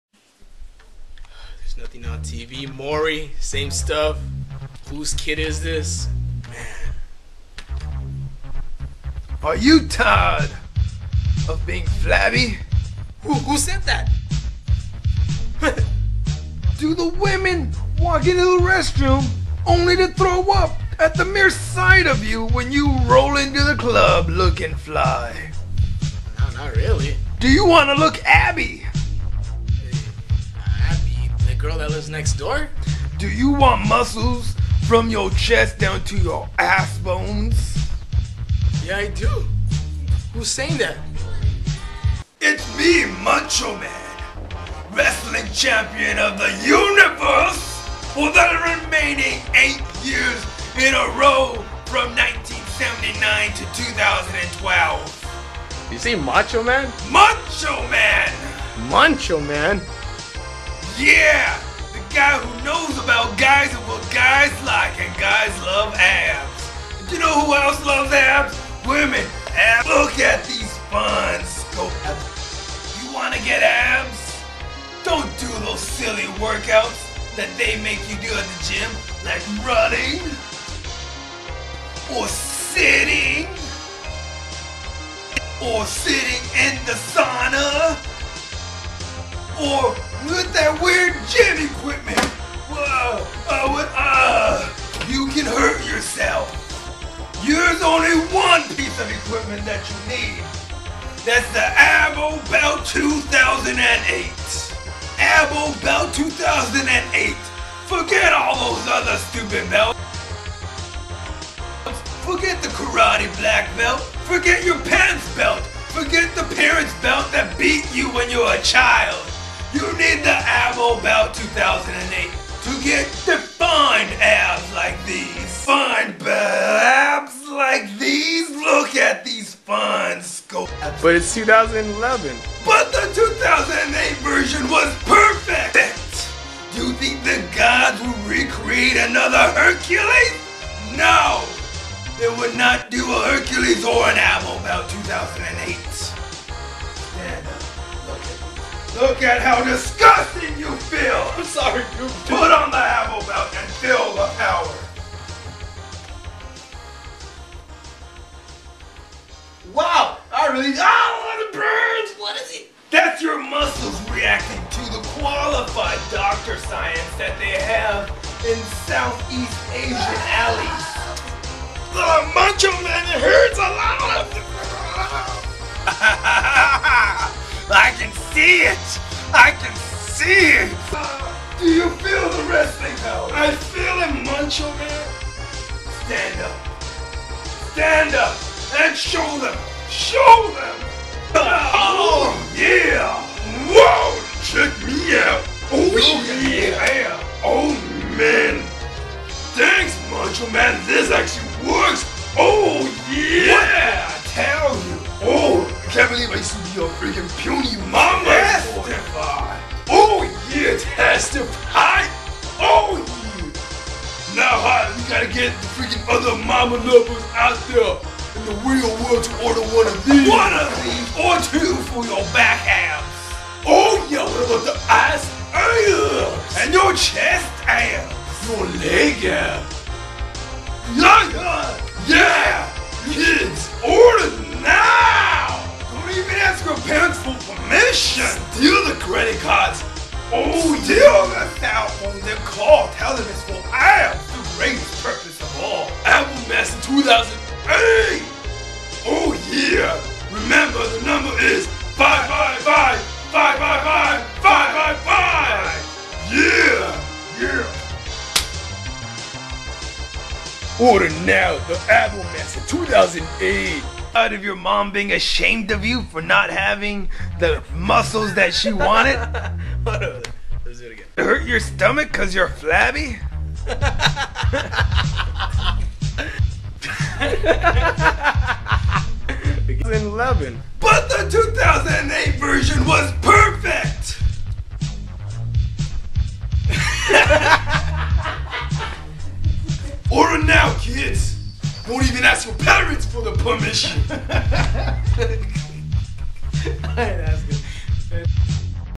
ABO BELT MASTER
ME MARTIN RIZO AND JOHNNY DOING A LITTLE INFOMERCIAL FOR ABO BELT 2008